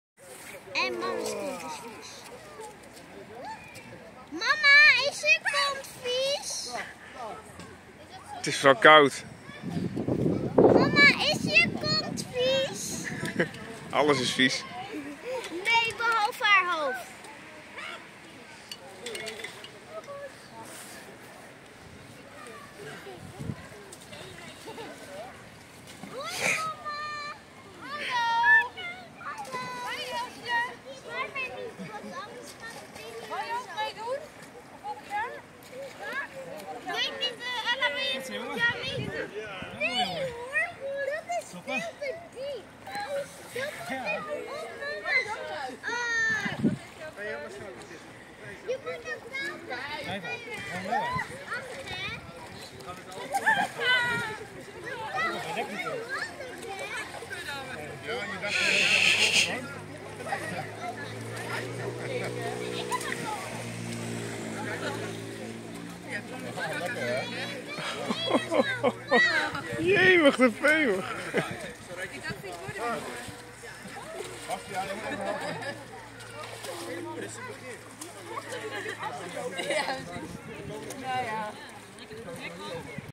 Kardingerun 2023 - Banjeren door de sloot
Kardinge run/ mudrun in Groningen bij Kardinge, Kardingermeer, zilvermeer